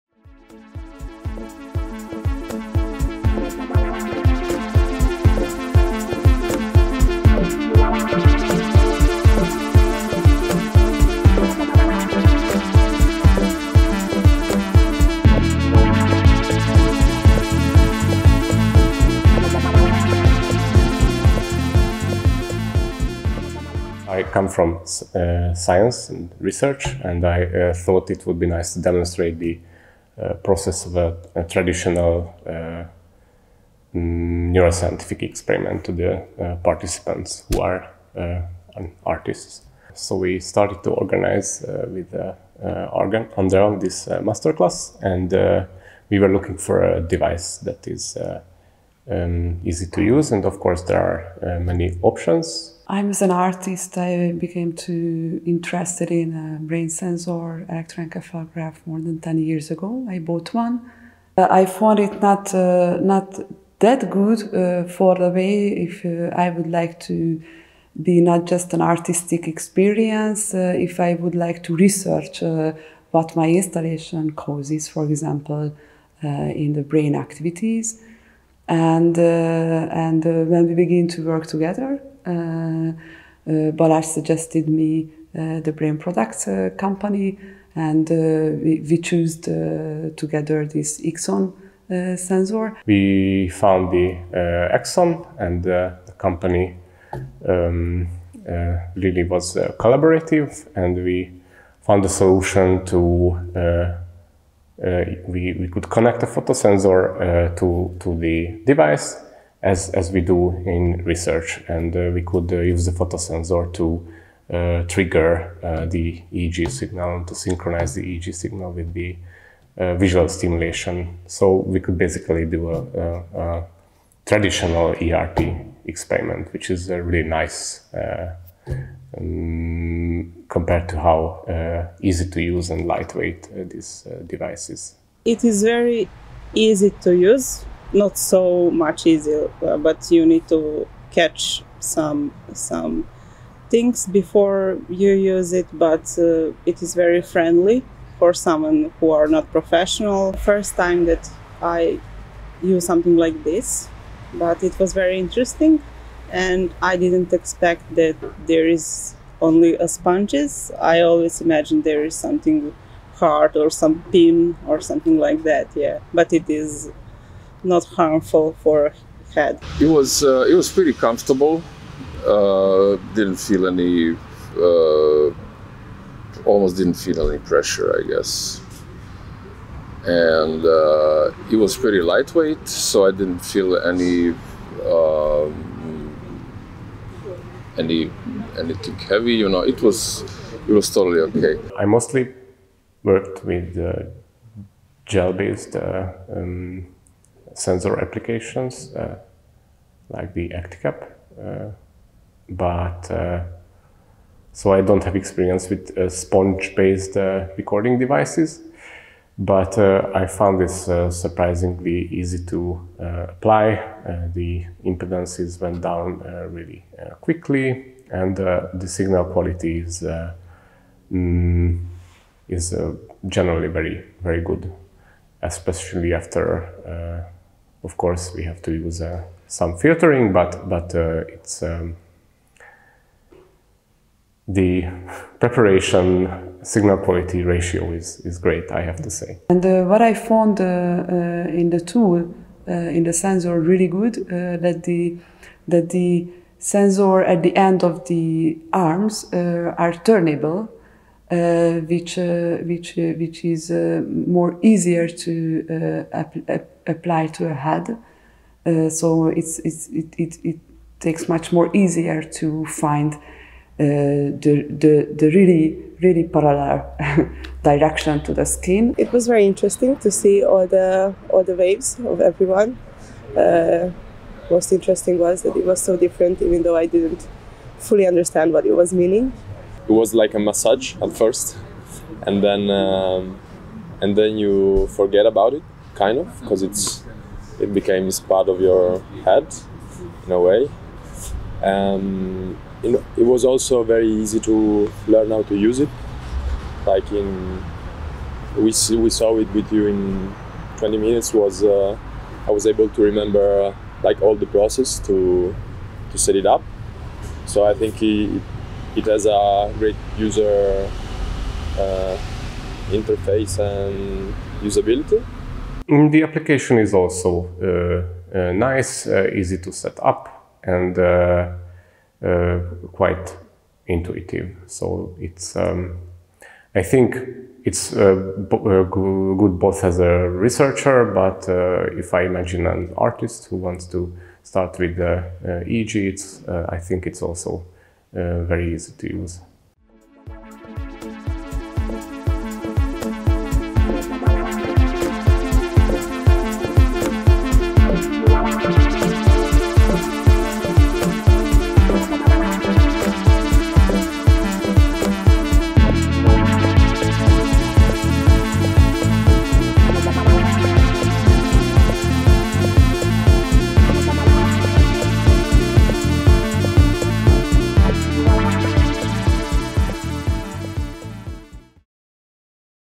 Experiments in neurodata-driven immersive visual art using the X.on EEG headset
IMMERSIVE SPACE | IMMERSIVE MIND summer masterclass
Light Art Museum Budapest LAM Masters

The ‘IMMERSIVE SPACE | IMMERSIVE MIND’ LAM masterclass was a one-week course centered on the use of EEG data as a basis of a visualisation set in the unique, zeppelin-shaped immersive projection environment of Light Art Museum Budapest.

This video documents the use of the X.on headset provided by Brain Products to record the EEG data that was the basis of the artistic process on the masterclass.

Video and motion graphics: Andrea Sztojánovits, Balázs Knakker
Recording: András Szijj

Masterclass project leader, organizer: Andrea Sztojánovits
Masterclass project coordinator: Eszter Gyöngyösi

Masterclass instructors:
Neuro Lab: Balázs Knakker (HU), Andrea Sztojánovits (HU)
Data Lab: Gábor Kitzinger (HU), Machiel Veltkamp (NL)
Space Lab: Viktor Vicsek (HU), Takeshi Yamada (JP)

Special thanks to Tamás Stolmár (Qualitis)